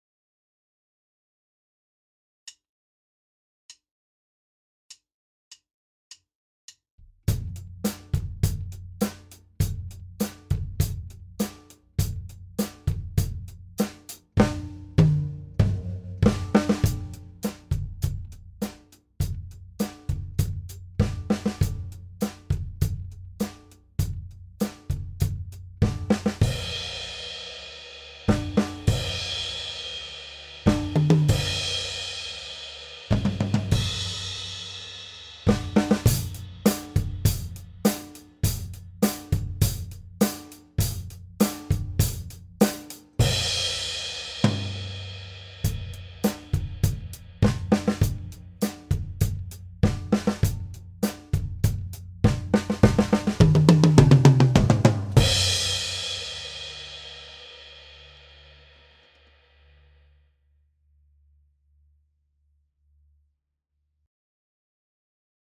london college of music drumkit exams grade two pick up the beat
I played the songs of London College of Music drumkit exams to help the students